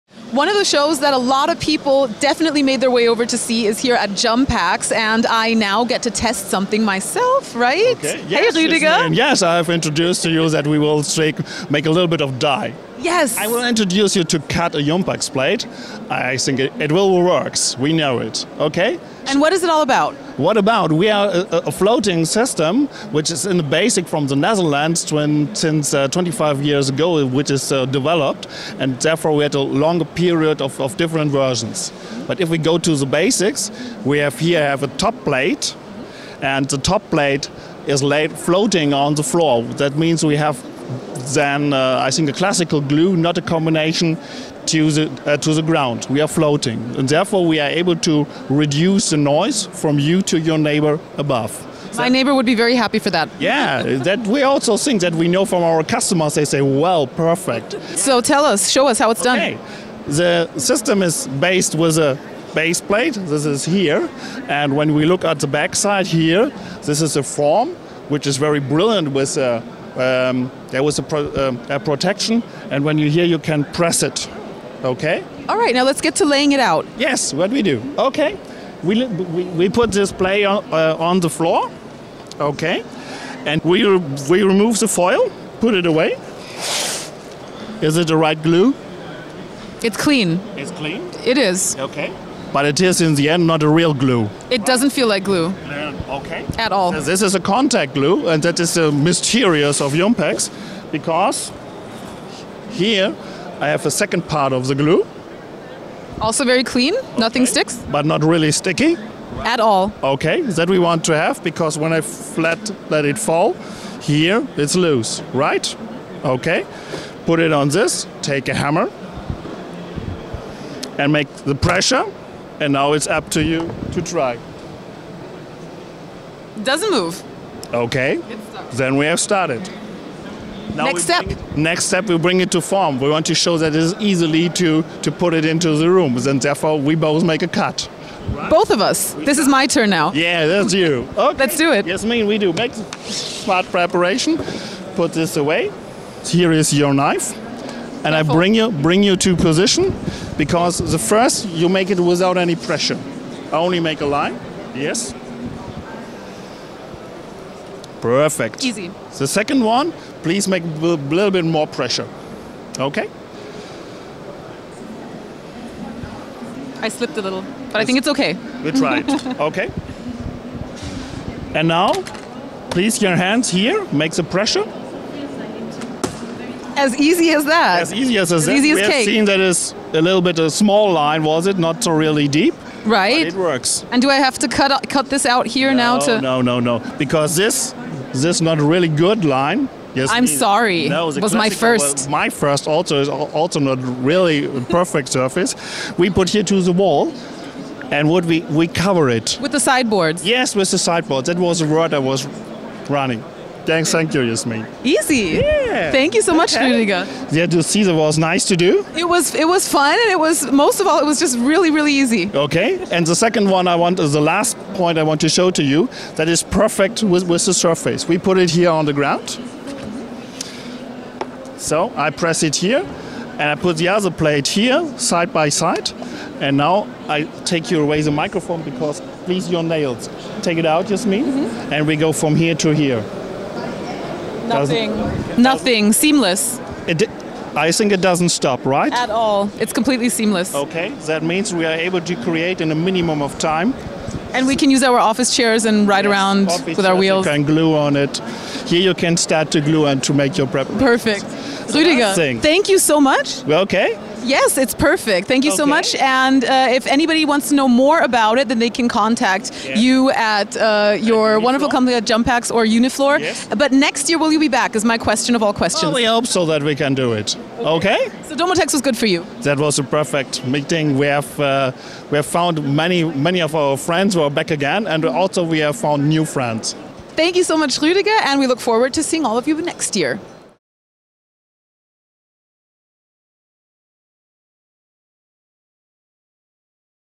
DOMOTEX TV - Unifloor
DOMOTEX TV spoke with the exhibitor Unifloor.